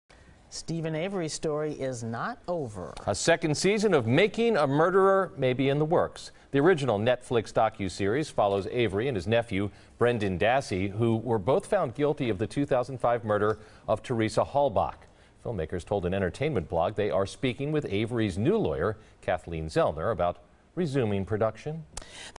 "Making a Murderer" Season 2?
Avery's new attorney Kathleen Zellner could be a part of a second season